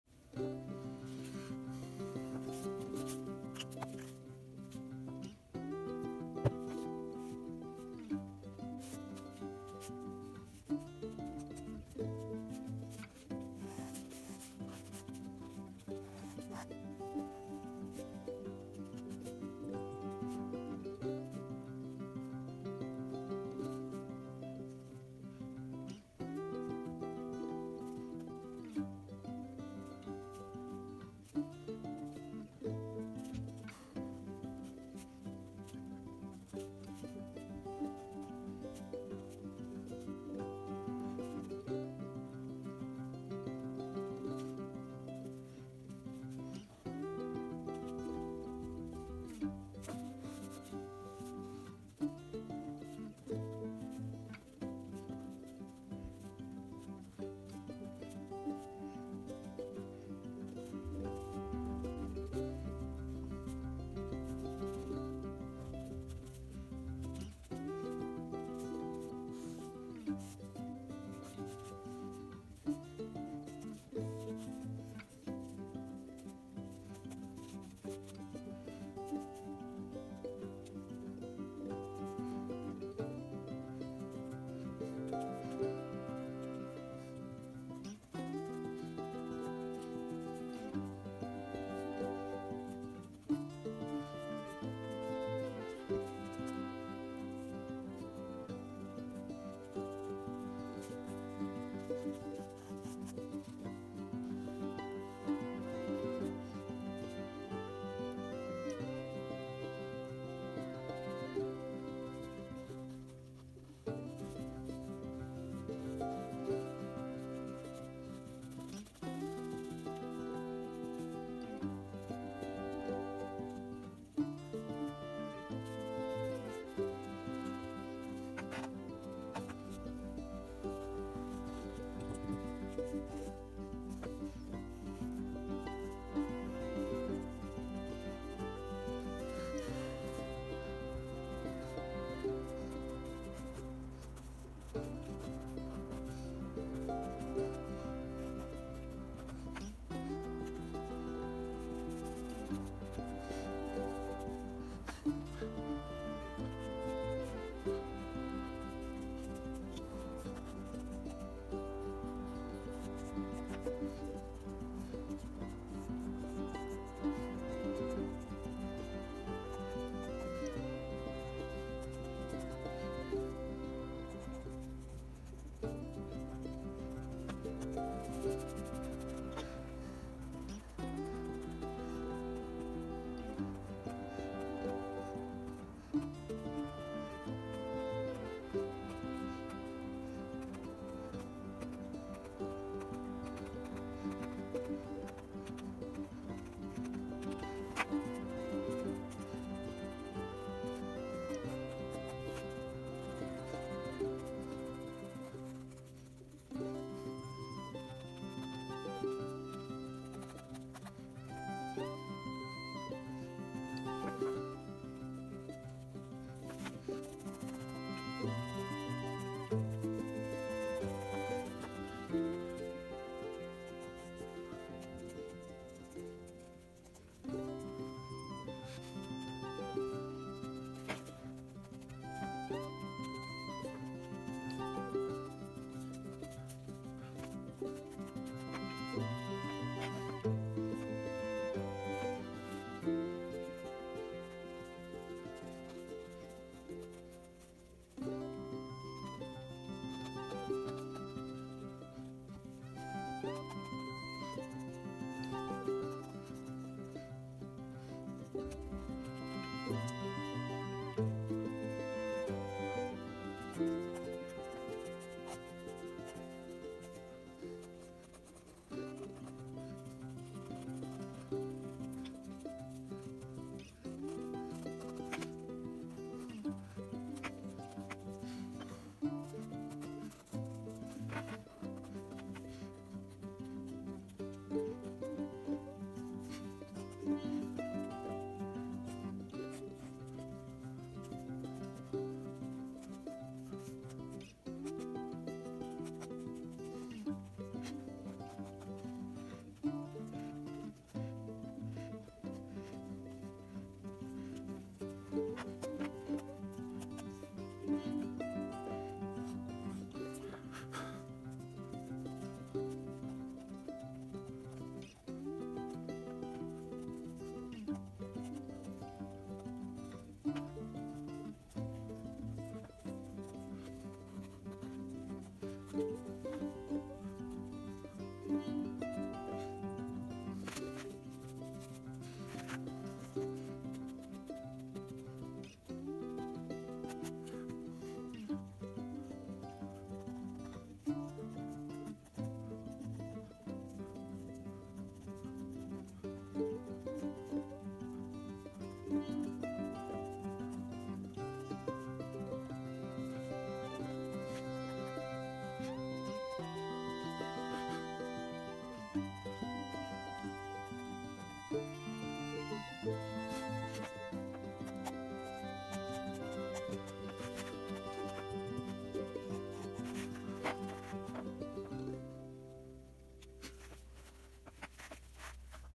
New “flexi-folded” nib design.
I did the tall ship sketch with v.1 but for this old factory sketch, I’m using v.2 (I made a new flexi-folded nib and fitted it into an old pen with a broken tip).
I’m really enjoying the line variation, and using brass for the writing tip makes it very smooth on the paper.